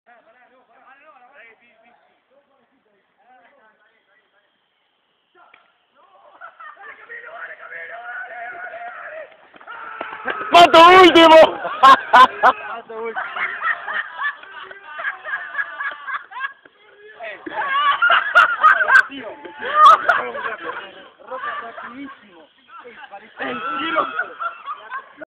Carrera de orkos
En el chako, la competencia 30 metros llanos en havaianas o descalzos a eleccion, Lassaga apostaste y perdiste, como siempre ! jaja